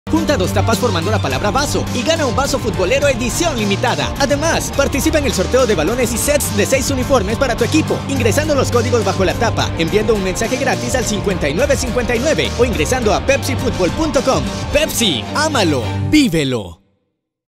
Vasos coleccionable PEPSI. ¡Mira como puedes coleccionarlos!
¿Ya tienes tu vaso coleccionable de PEPSI? Mira el video y conoce cómo puedes coleccionarlos todos.

Con tu PEPSI en las presentaciones de 500ml y 3 Litros no retornable deberás formar la palabra VA + SO con dos (02) tapas contramarcadas. Los vasos pueden ser canjeados en los camiones repartidores o en tu lugar de compra favorito, debes presentar las dos (02) tapas contramarcadas formando la palabra VA + SO

*Recuerda conservar tus taparroscas para canjear tus premios.